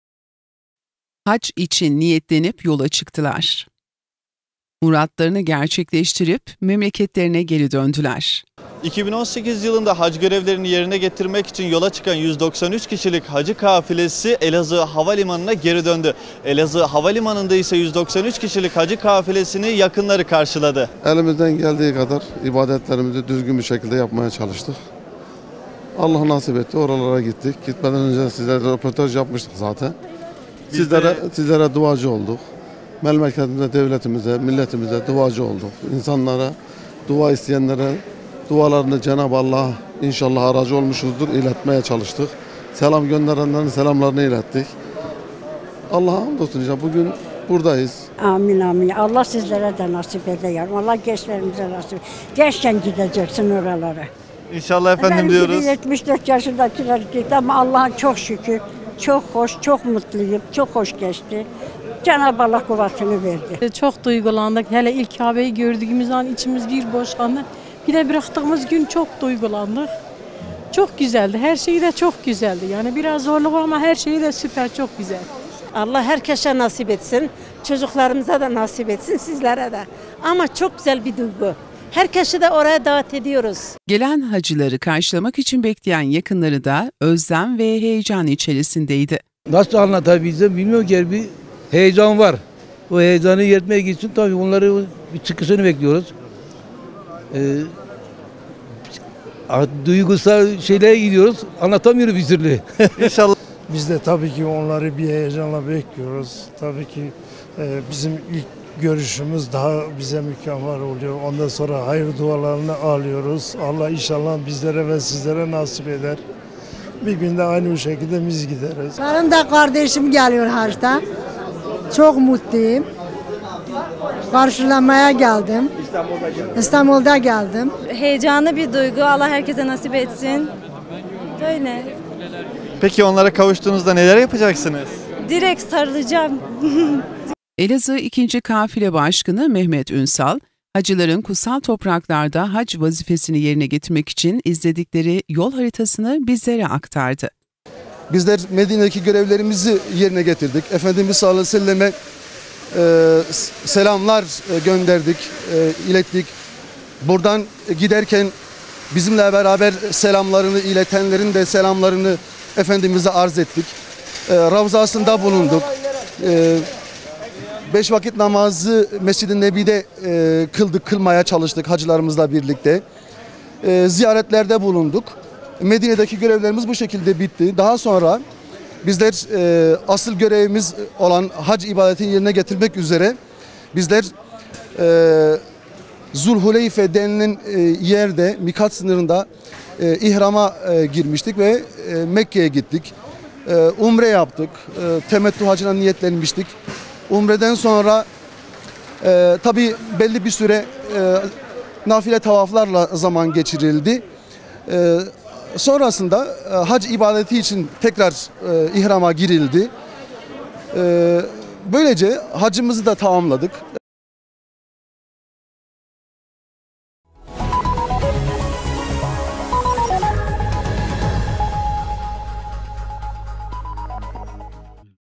Elazığ'da 193 Kişilik Son Hacı Kafilesi Geri Döndü
Hac görevlerini yerine getirmek üzere yola çıkan son hacı kafilesi görevlerini yerine getirip kutsal topraklardan memleketlerine geri döndüler.